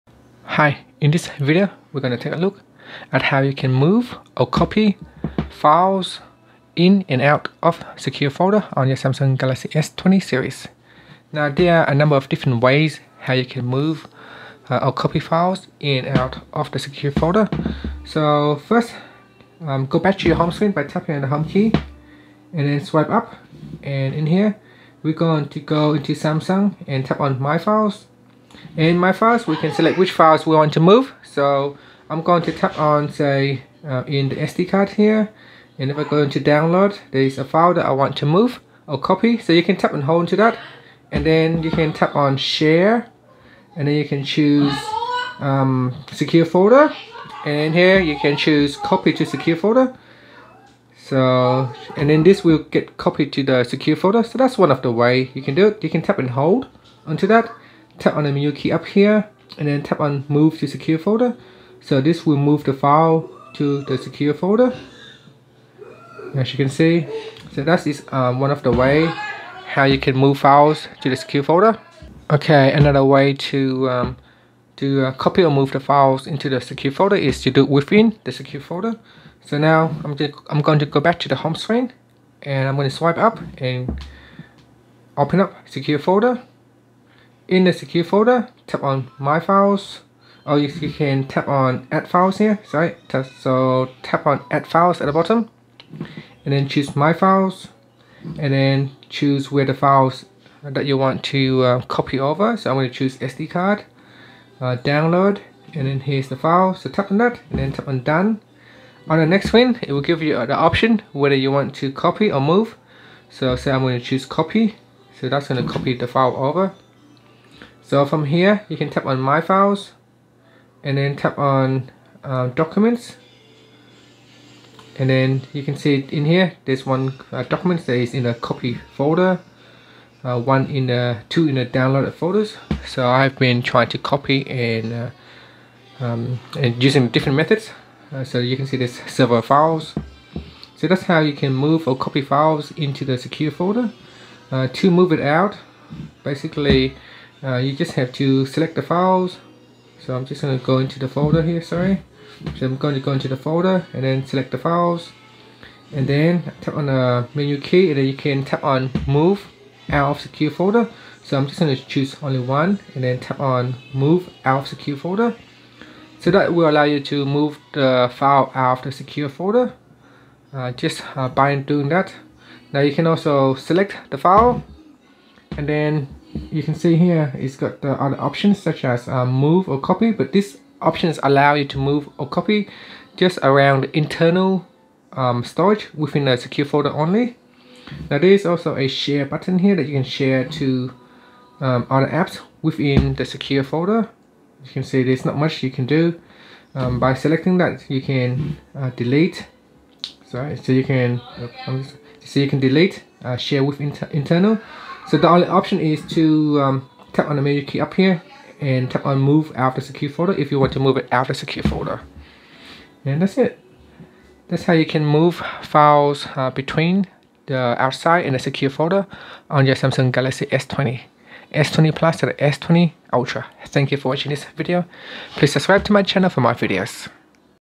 Galaxy S20/S20+: How to Move / Copy Files In / Out of a Secure Folder
Learn how you can move or copy files In / out of a Secure Folder on Galaxy S20 / S20 Plus / S20 Ultra.

Android 10.